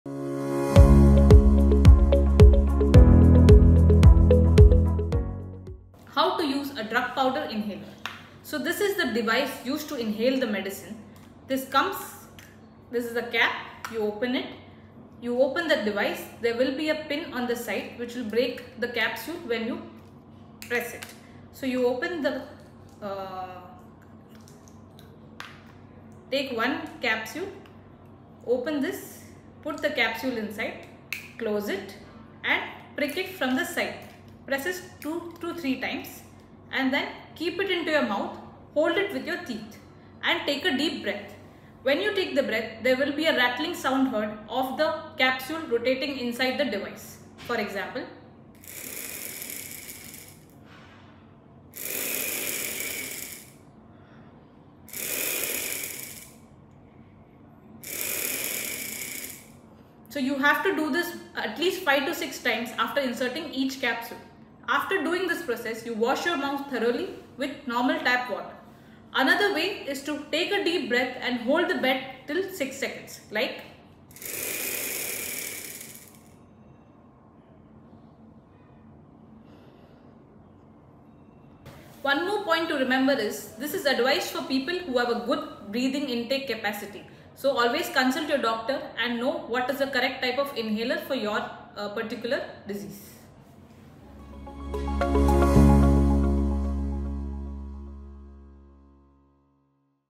KMC Hospital Mangaluru | Dry Powder Inhalers: How to Use Them | Dr. Udaya Sureskumar
Dry powder inhaler is a type of inhaler that allows the medication to go deep into the lungs. In this informative video, Dr. Udaya Sureshkumar, Consultant - Respiratory Medicine & Pulmonology, shares some important information about the correct use of dry powder inhalers.